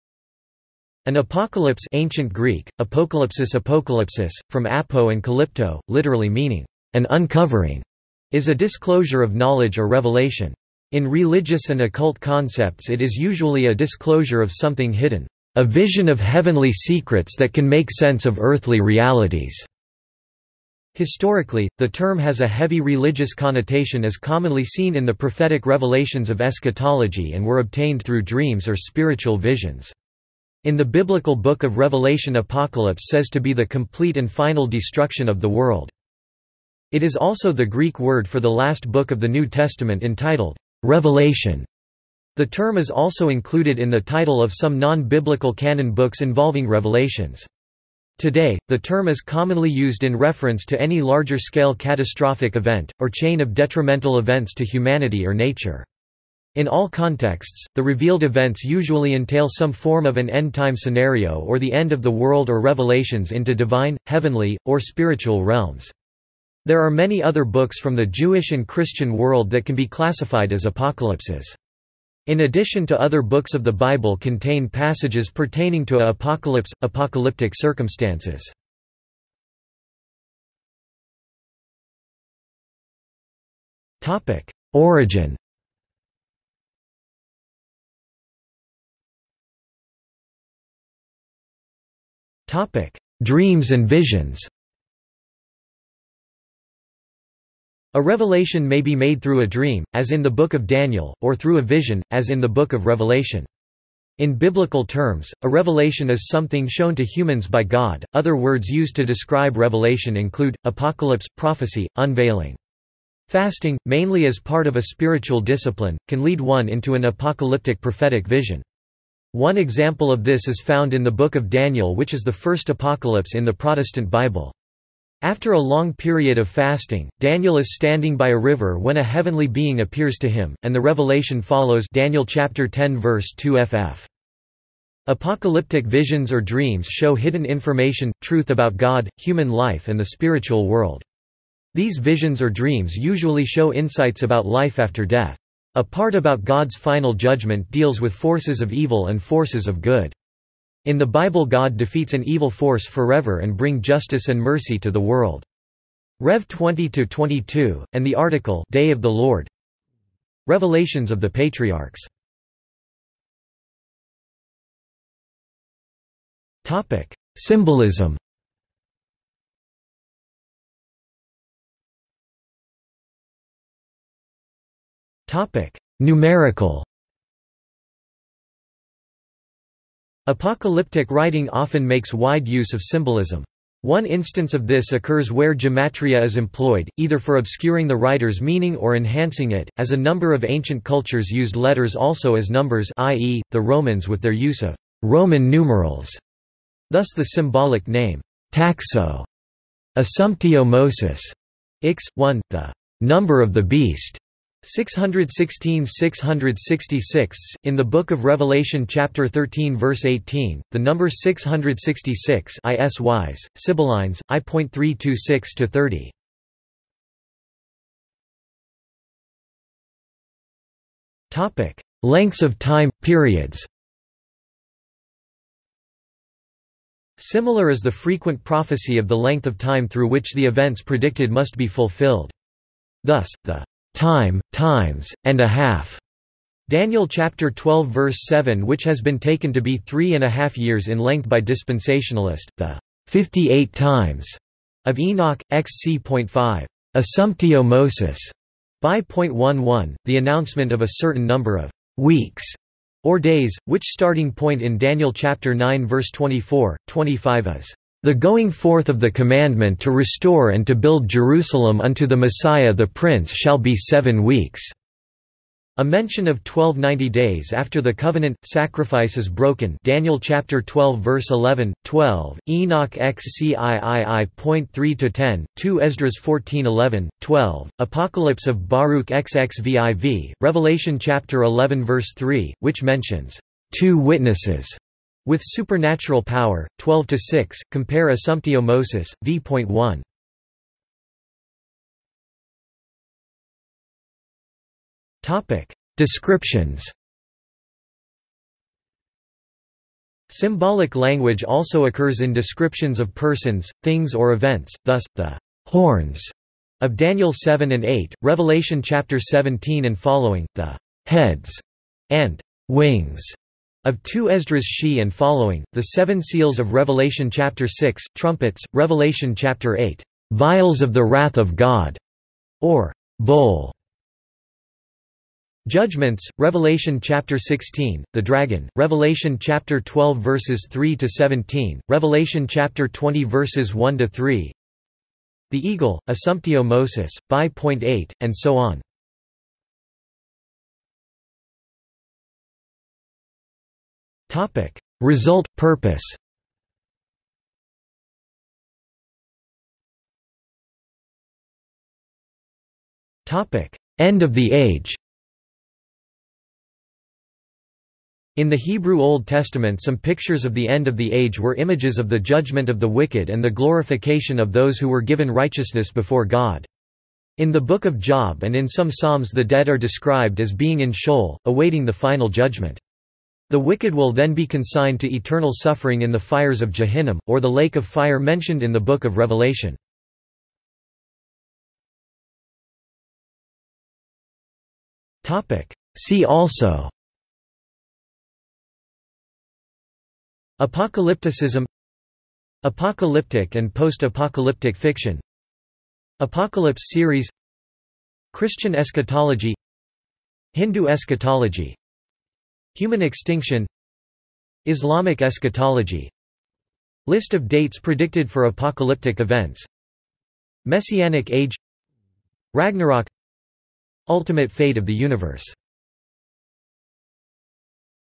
Apocalypse | Wikipedia audio article
This is an audio version of the Wikipedia Article:
Apocalypse

00:01:20 1 Origin
00:01:29 1.1 Dreams and visions
00:02:43 2 Symbolism
00:02:51 2.1 Numerical
00:03:38 2.2 Lengths of time / periods
00:04:51 2.3 Descriptions
00:05:40 3 Result/purpose
00:05:49 3.1 End of the age
00:06:21 4 See also

- Socrates

SUMMARY
An apocalypse (Ancient Greek: ἀποκάλυψις apokálypsis, from ἀπό and καλύπτω, literally meaning "an uncovering") is a disclosure of knowledge or revelation. In religious and occult concepts it is usually a disclosure of something hidden, "a vision of heavenly secrets that can make sense of earthly realities". Historically, the term has a heavy religious connotation as commonly seen in the prophetic revelations of eschatology and were obtained through dreams or spiritual visions. In the biblical book of revelation apocalypse says to be the complete and final destruction of the world.
It is also the Greek word for the last book of the New Testament entitled "Revelation". The term is also included in the title of some non-biblical canon books involving revelations. Today, the term is commonly used in reference to any larger-scale catastrophic event, or chain of detrimental events to humanity or nature. In all contexts, the revealed events usually entail some form of an end time scenario or the end of the world or revelations into divine, heavenly, or spiritual realms. There are many other books from the Jewish and Christian world that can be classified as apocalypses. In addition to  other books of the bible contain passages pertaining to a apocalypse/ apocalyptic circumstances.